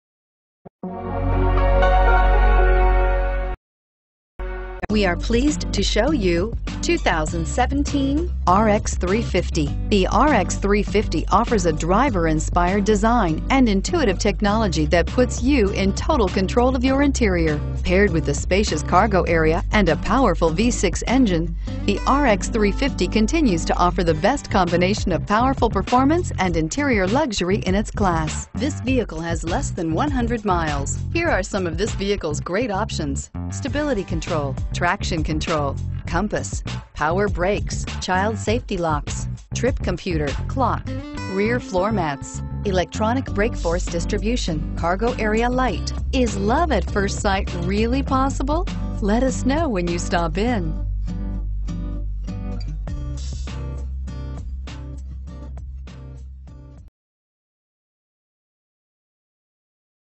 2017 Lexus RX 350 for sale in Raleigh NC
2017 Lexus RX 350 for sale in Raleigh NC - Stock # 39035
Year: 2017
Engine: 3.5L V6 DOHC 24V
Transmission: Automatic
Exterior: Eminent White Pearl
Interior: Parchment Leather With Matte Bamboo Trim
Drive Train: FWD
Cylinders: 6
Trim: N/A
Location
Address:
VIN: 2T2ZZMCA6HC087411